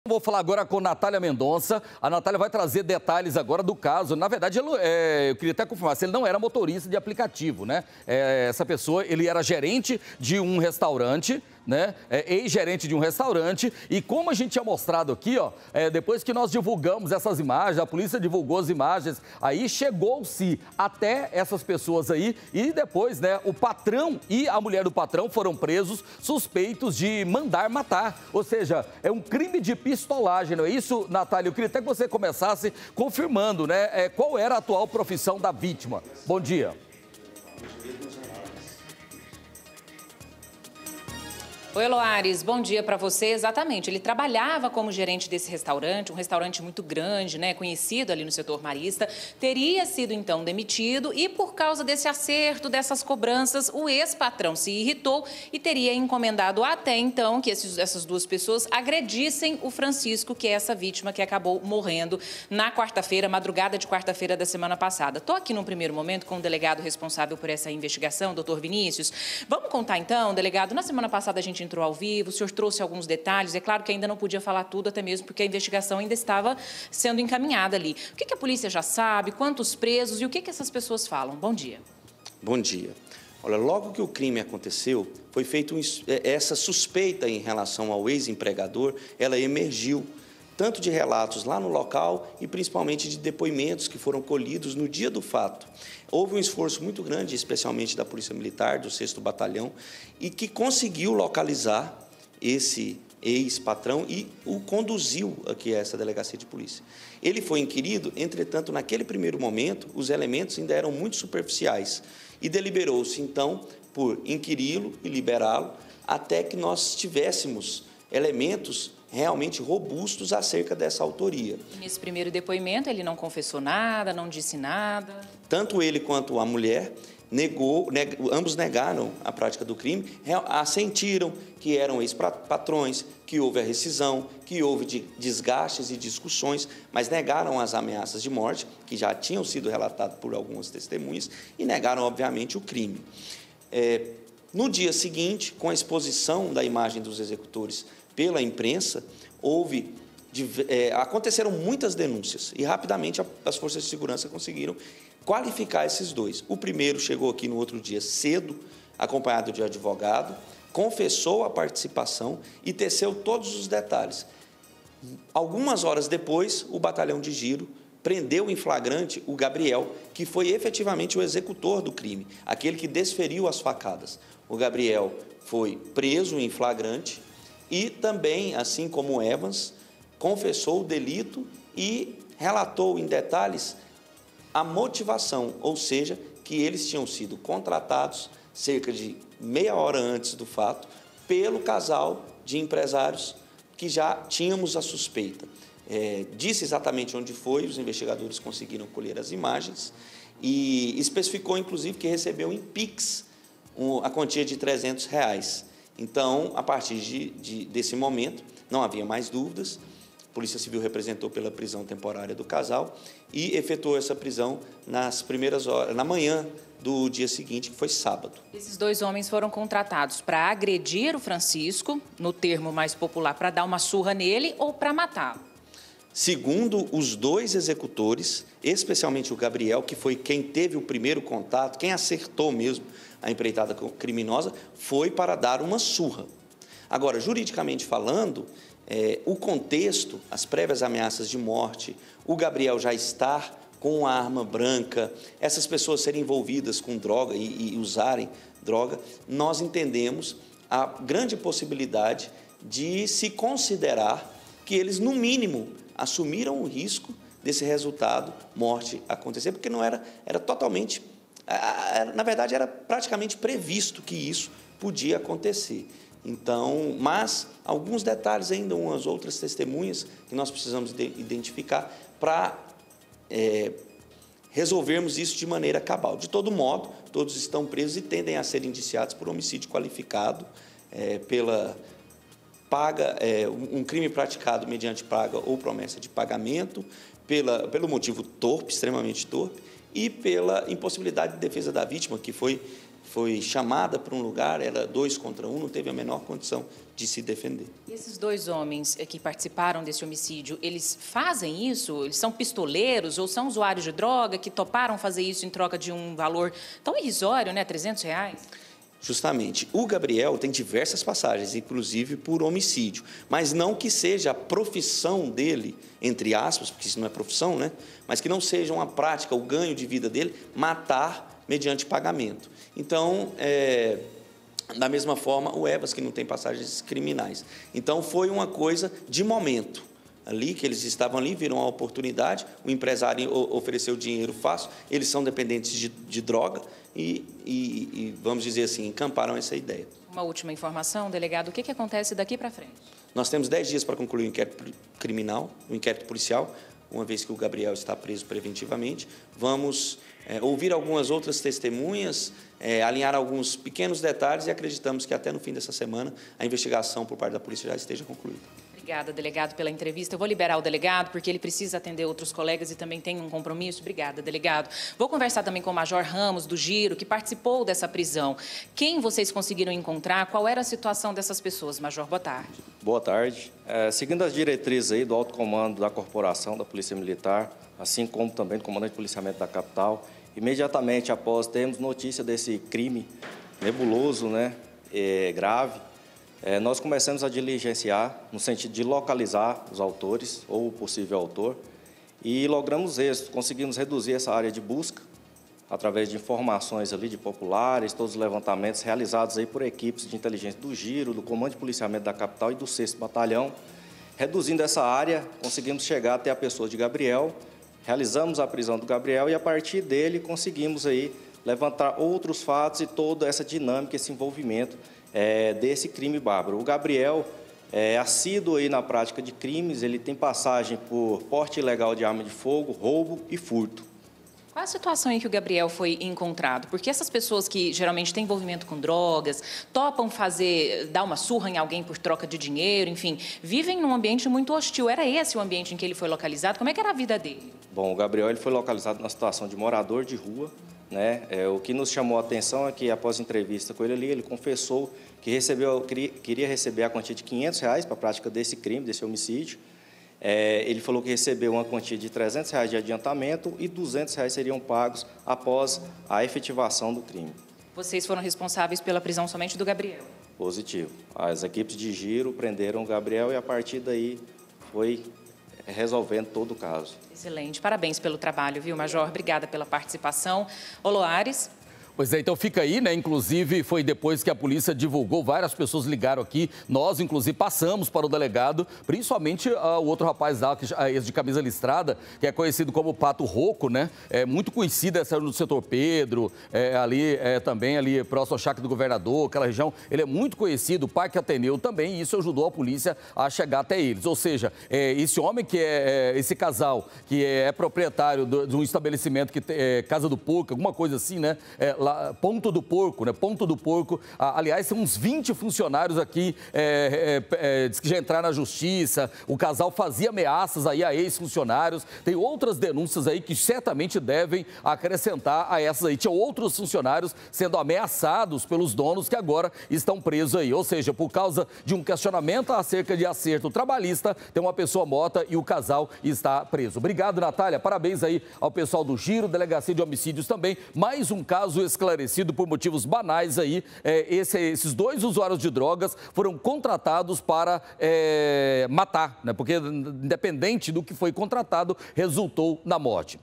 MORTE NO JARDIM GOIÁS: PATRAÕ É O SUSPEITO DE MANDAR MATAR EX-FUNCIONÁRIO
A NATHÁLIA TRAZ AGORA ATUALIZAÇÃO DO CASO DO MOTORISTA DE APLICATIVO MORTO NO JARDIM GOIÁS. NATHÁLIA, O PATRÃO É O SUSPEITO DE MANDAR MATAR EX-FUNCIONÁRIO?